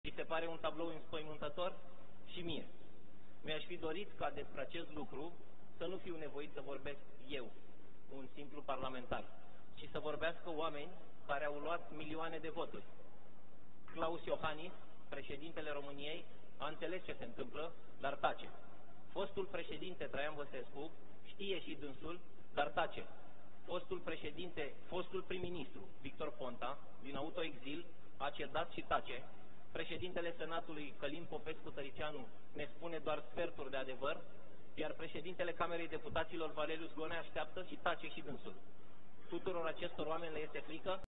Ședința în plen a Camerei Deputaților României din 30 martie 2016
Sebastian Ghiță: Oamenii cu milioane de voturi tac în fața dosarelor DNA.
Titlul evenimentului: Ședința în plen a Camerei Deputaților României din 30 martie 2016